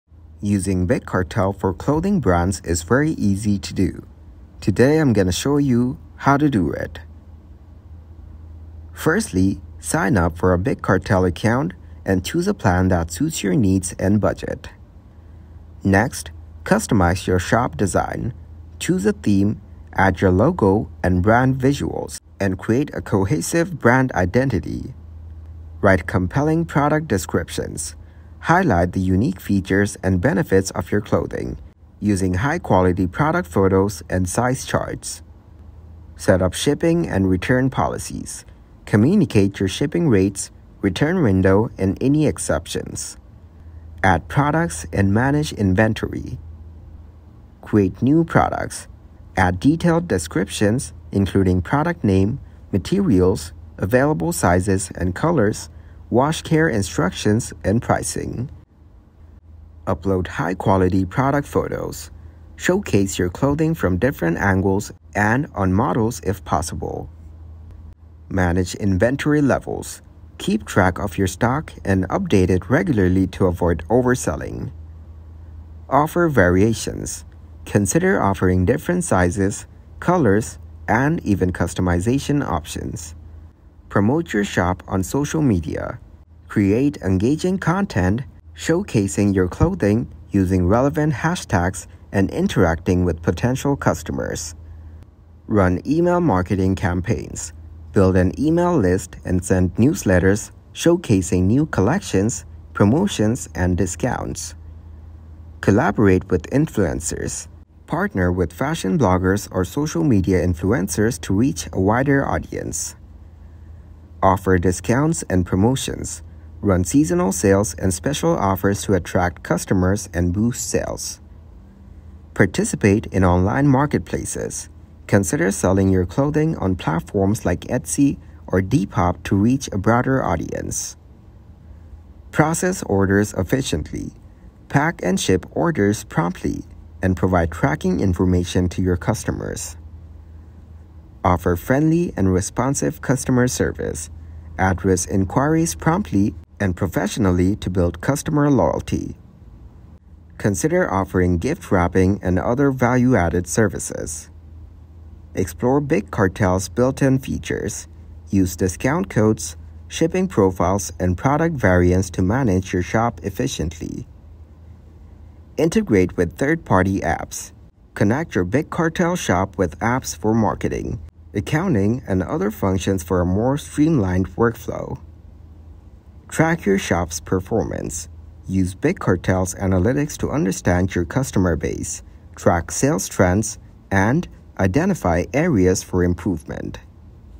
HOW TO USE BIG CARTEL FOR CLOTHING BRAND 2025! (FULL GUIDE)
Welcome to a game-changing guide on "How to Use Big Cartel for Your Clothing Brand." In this comprehensive video, we unveil the secrets to navigating Big Cartel, ensuring your brand's success in the competitive world of fashion.

0:00 INTRO
0:04 TUTORIAL